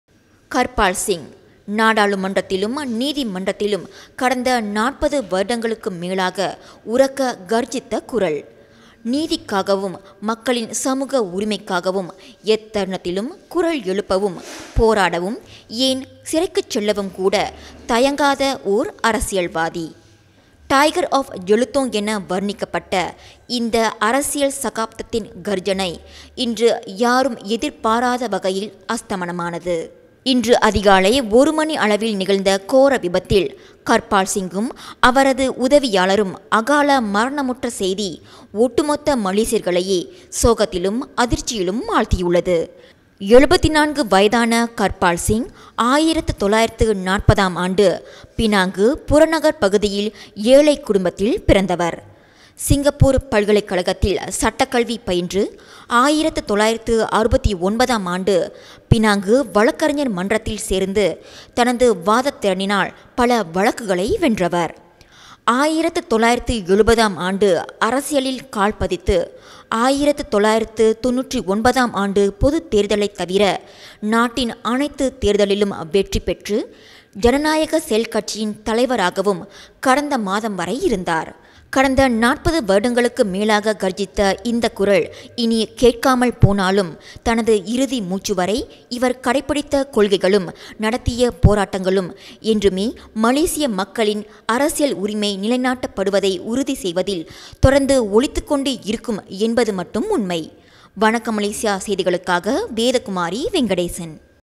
1940-2014: fearless “Tiger of Jelutong” Karpal Singh
நாடாளுமன்ற சிங்கம், நாடறிந்த வழக்குரைஞர், மூத்த அரசியல்வாதி, நாடாளுமன்ற உறுப்பினர் என பன்முகங்களில் நாட்டைக் கலக்கி வந்தவர் அஞ்சா நெஞ்சர் கர்பால் சிங். 1940-ஆம் ஆண்டு பிறந்த இவர், இன்று அதிகாலை நிகழ்ந்த சாலை விபத்தில், காலமானார்.லும்